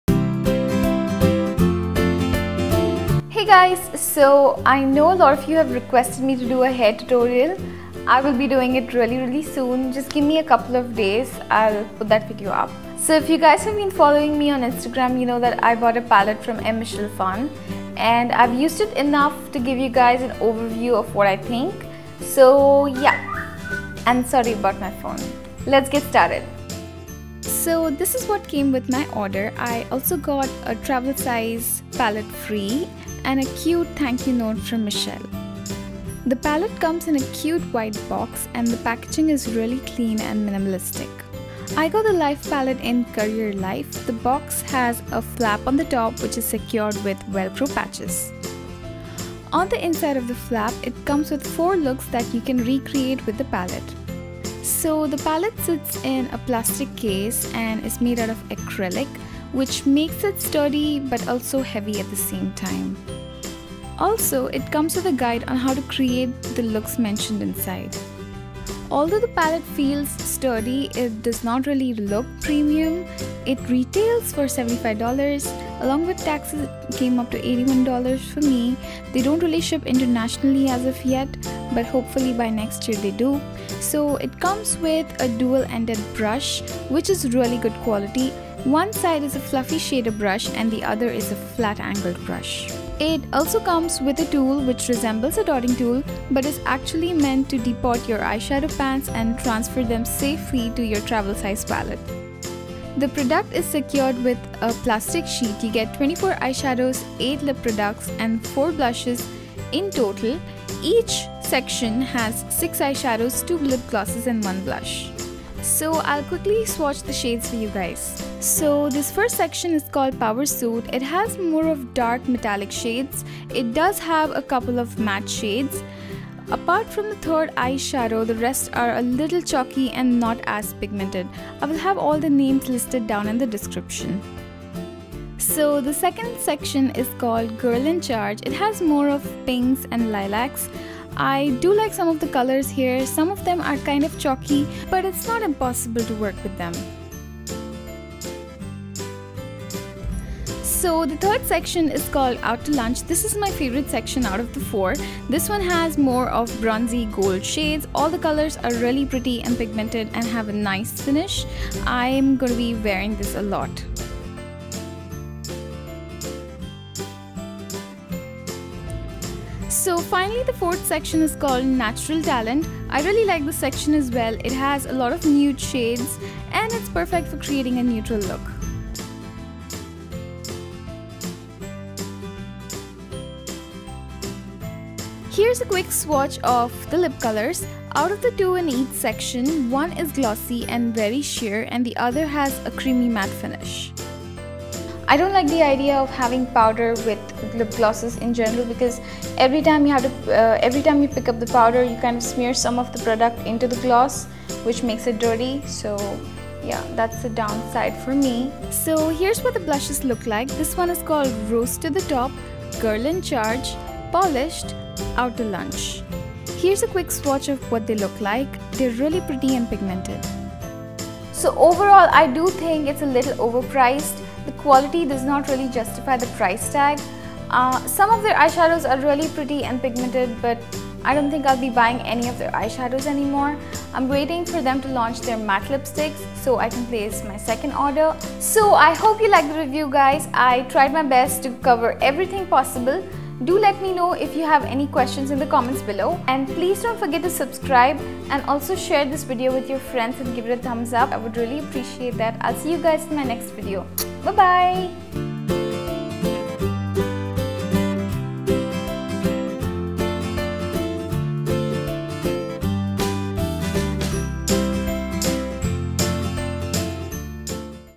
Em Cosmetics by Michelle Phan - Career Life Palette Review
Here's my Michelle Phan Em Cosmetics Career Life Palette Review Video

I'm an Instagram junkie! :D

I post a lot on my facebook page too. You don't wanna miss it! :P

Come, lets hangout! ( google + )

Look 1: Power Suit

Eyeshadows:
String of pearls
Sheer ambition
Silk blouse
Pencil skirt
Black pumps

Lips:
Power suit
Bold ststement

Blush:
Rose to the top

Look 2: Girl in charge

Eyeshadows:
Pinc inc.
Conference call
Madison ave
Seriously
Corner office
Corporate card

Lips:
Chic
Posh

Blush:
Girl in charge

Look 3: Out to lunch

Eyeshadows:
Tgif
Inbox: full
Running late
Olive
Espresso bean
Meet me in the lobby

Lips:
Dynamic
Nine to five

Blush:
Out to lunch

Look 4:

Eyeshadow:
Paperwork
Natural talent
Take a break
Nude rose
On the grind
32nd Floor

Lips:
Dressed up
Vip girl

Blush:
Polished

em cosmetics india